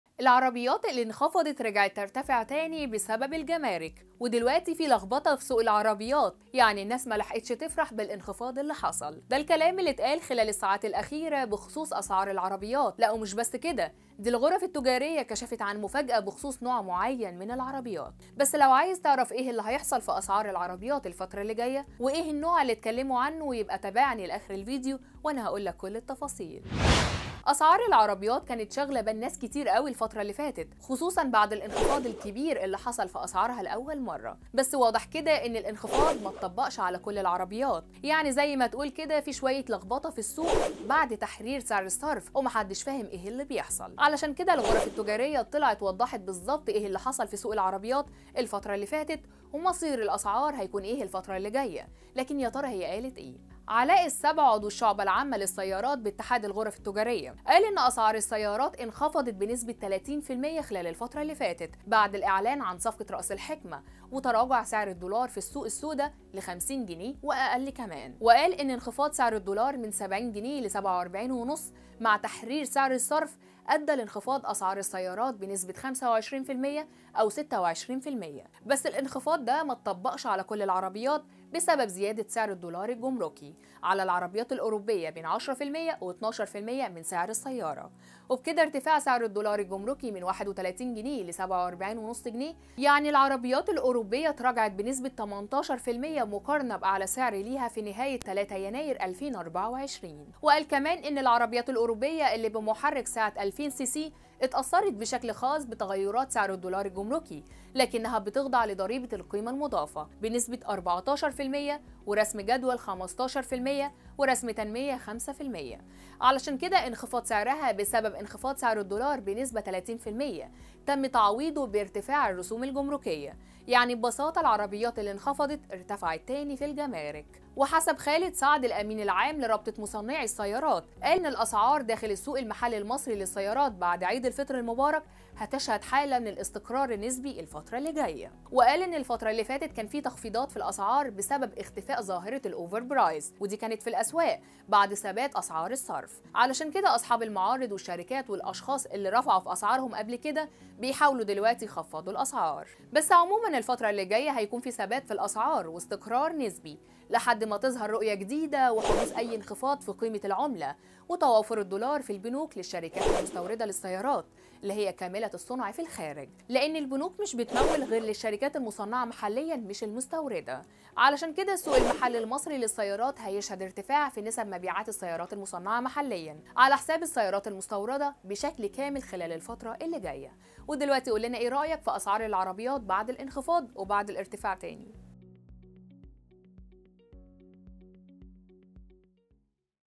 جنون سوق السيارات.. الأسعار رجعت تزيد تاني.. ومفاجأة بالجملة الفترة الجاية.. وهذه اخر الأسعار!
وقال أن انخفاض سعر الدولار من 70 جنيه ل47.5 جنيه مع تحرير سعر الصرف؛ أدى لانخفاض ‏أسعار السيارات بنسبة 25% أو 26‏‎%‎، بس الانخفاض ده مطبقش على كل العربيات  بسبب ‏زيادة سعر الدولار الجمركي على العربيات الأوروبية بين 10% و12‏‎%‎‏ من سعر السيارة